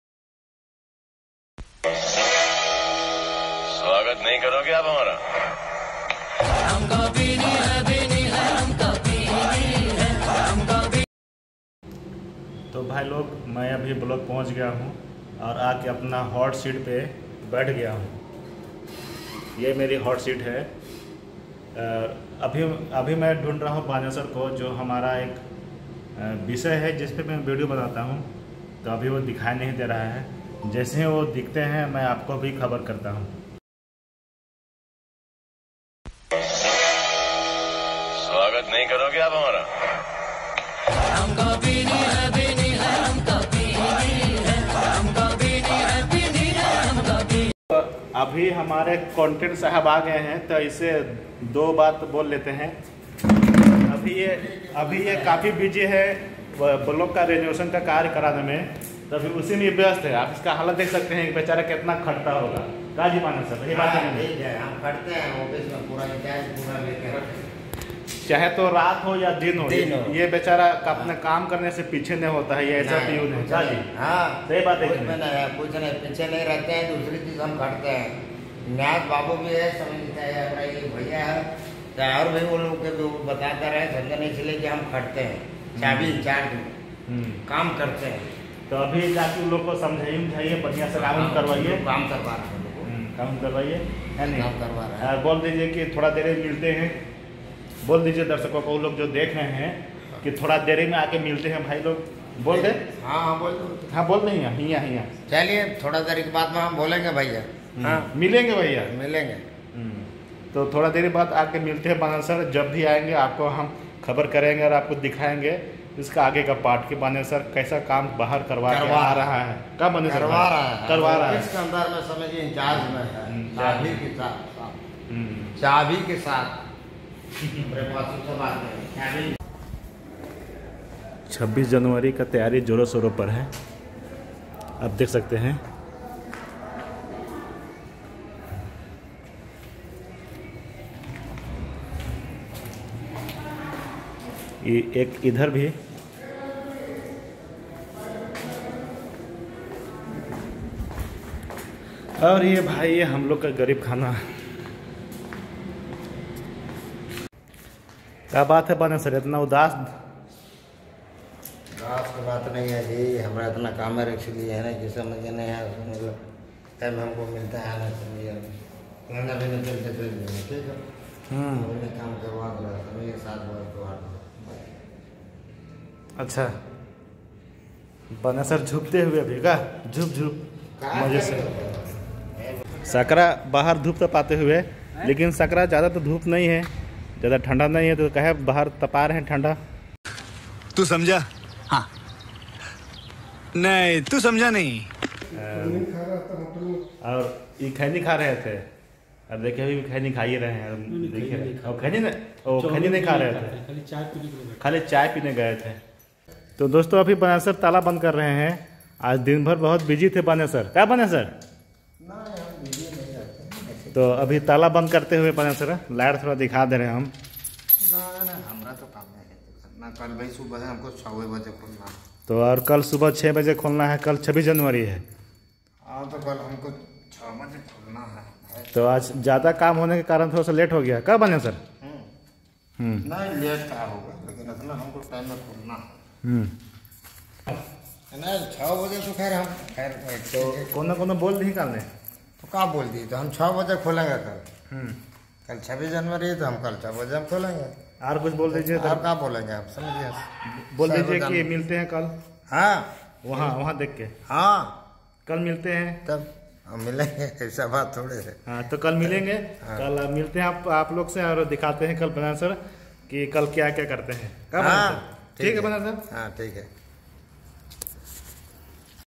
Baneshwar Aaj kal bahut busy aur preshan hai kaam ko lekar
यह वीडियो मात्र मनोरंजन परपस से बनाया गया है